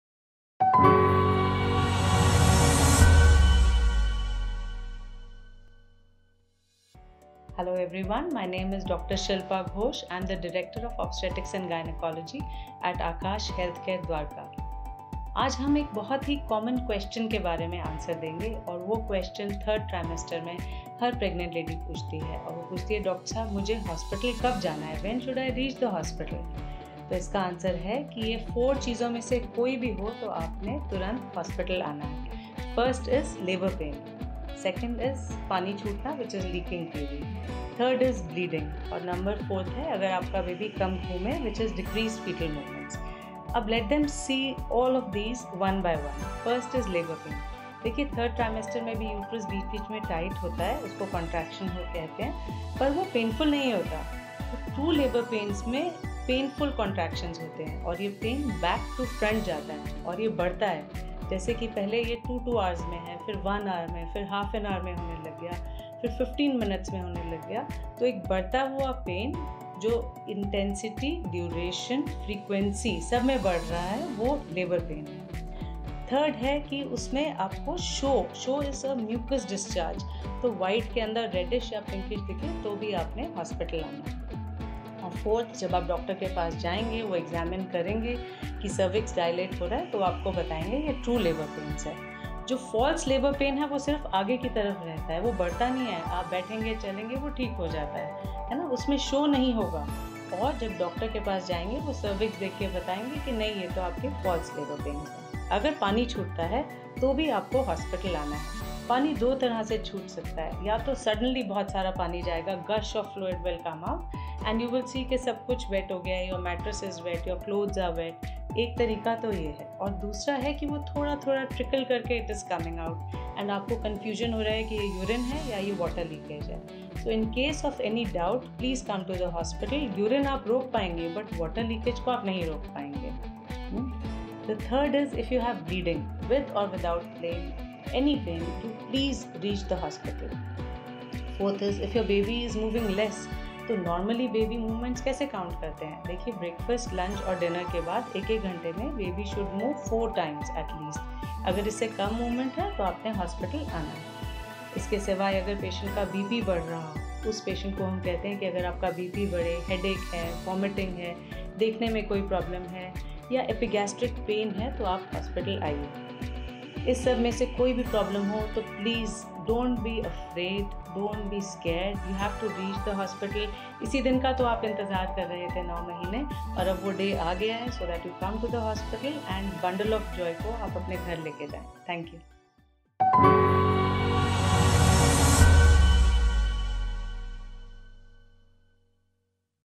डिलीवरी के लिए अस्पताल कब आऐं। | Dr. Shilpa Ghosh at Aakash Healthcare
► Today, we’ll answer a very common question, and that question is asked by every pregnant woman during each trimester. They ask “doctor when should I reach the hospital?”
The answer is if either of these four things happen: they have to get to a hospital.

► First is labour pains, the second is “paani tootna” which means leaking baby, the third is bleeding and number four is in case of decreased foetal movements.

► Now let’s take these one by one.

► First is labour pain, you must understand that the uterus does tighten now and again during the third trimester: called contraction but it is not painful unlike true labour pains which has painful contractions. This pain moves from back to front and keeps increasing. At first it may occur in a span of two hours, then one hour, after which it may begin every half an hour, and then on to every fifteen minutes. So this incremental pain that rises in intensity, duration, and frequency: is Labour pain.

► Then there’s show: a mucous discharge, so if you see white or red in it: you must come to the hospital immediately.

► And finally, when you visit the doctor, they will see whether the cervix is dilated: which will help them determine whether these are true labour pains or not.
Remember that false labour pains take place in the front, when you walk or sit: it’ll rectify itself, won’t have show, and when the doctor sees the cervix is not dilating: they will inform you that these are false labour pains.

► In case your water breaks, you still have to come to the hospital. Water can break in two ways, either a lot of water will be discharged suddenly like a gush of fluid and you will see everything getting wet; your mattresses will get wet, your clothes will get wet, etc.

► The other way water breaks is by trickling out bit by bit; you may get confused thinking if it’s urine or a water leakage.

► So in case of any doubt, please come to hospital. Remember thay you can stop the flow of urine but not water leakage.

► Now if you have bleeding with or without any pain: please reach the hospital.

► If your baby is moving less, now how do you measure if your baby is indeed moving less?
What you must observe: after breakfast, lunch and dinner: your baby should at least four (at least) times within an hour of these meals. Now if there is lesser movement than this, you need to get to the hospital.

► Apart from this, if a patient’s blood pressure is rising along with headaches, vomiting, trouble seeing clearly, or with epigastric pain: we advise you to come to the hospital as well.

► If you face any of these problems: please don’t be afraid, don’t be scared: you have to reach the hospital for this is the very day you’ve been waiting for nine months. So come to the hospital and take your bundle of joy back home.

Dr. Shilpa Ghosh Director & Senior Consultant – Obstetrics & Gynaecology at Aakash Healthcare Super Speciality Hospital, Dwarka